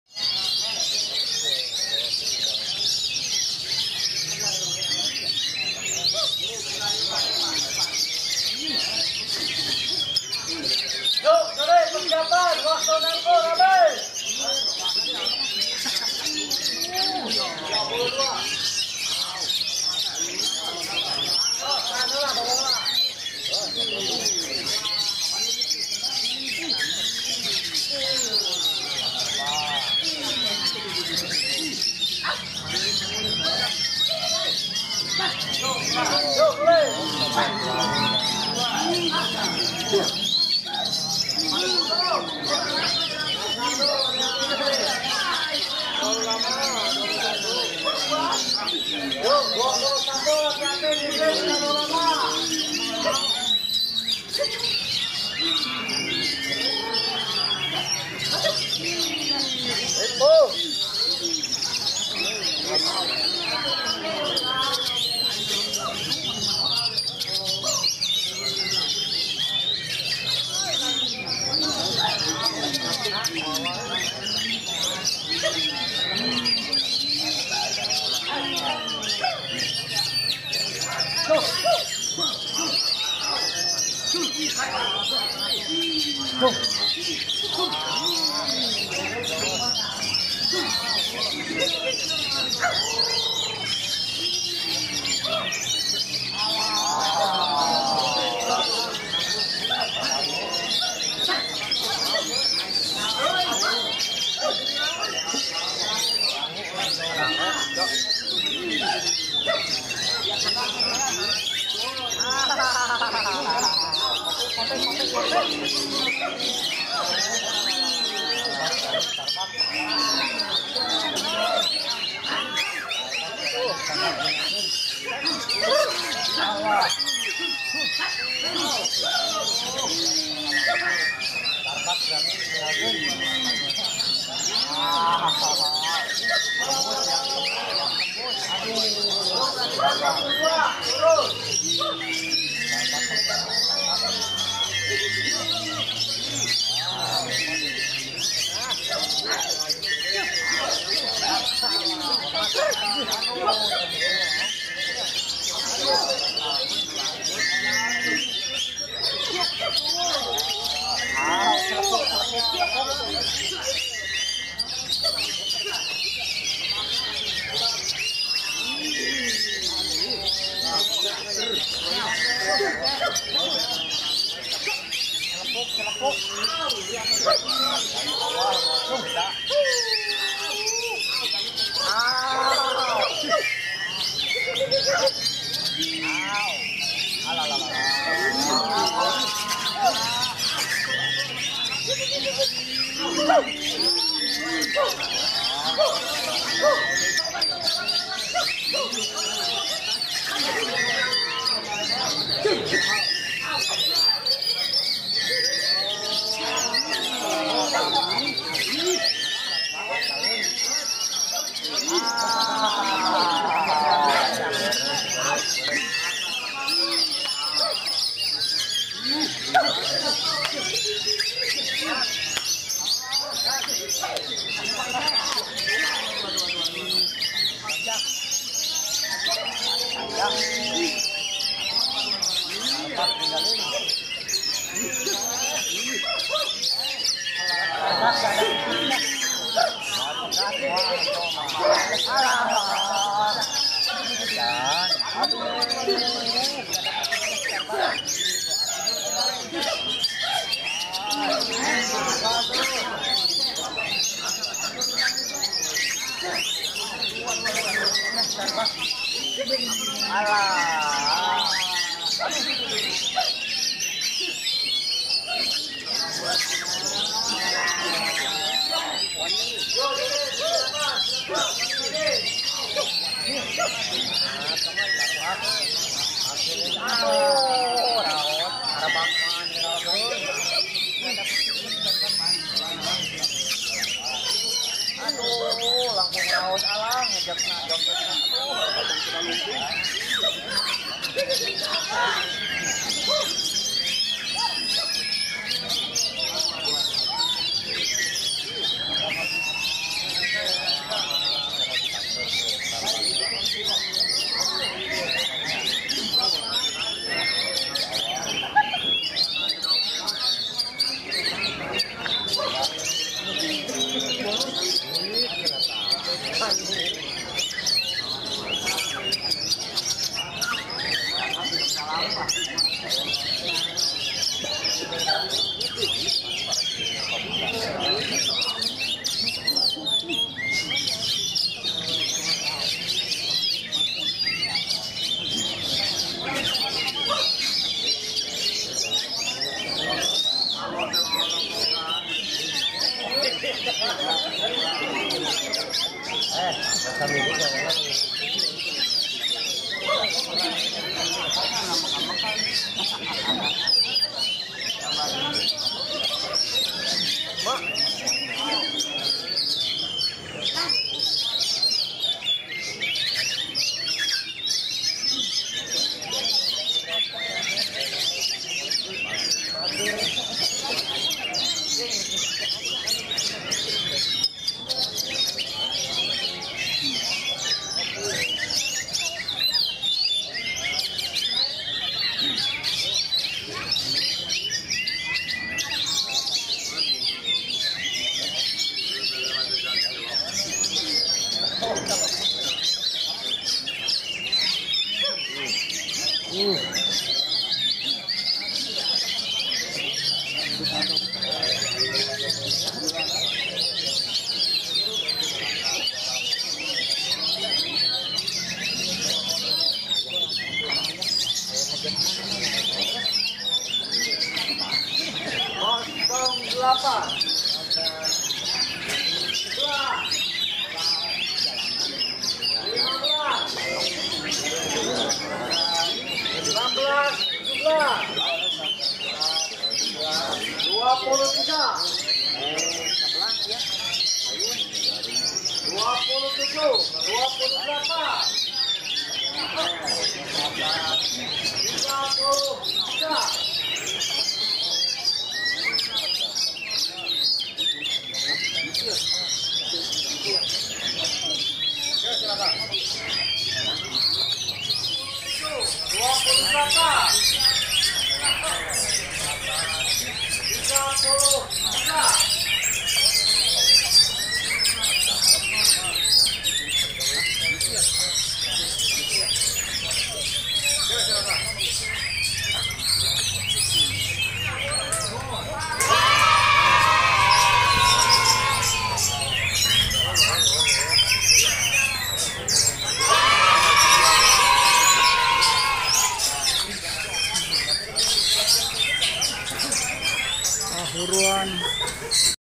Kacer Android - VRA SF Jatinangor ( KMB tanjungsari ) Hattrick Di Exclusive Panorama BC
Pada video kali ini menampilkan aksi Kacer yang bernama " ANDROID " gacoan milik VRA SF Jatinangor ( KMB Tanjungsari) yang berhasil tampil maksimal dan menjadi juara ke 1 di kelas wajib sangkar Ebod Jaya Variasi Terbaru dan juga tampil juara ke 1 di dua kelas lainnya ( Hattrick ).
Trims...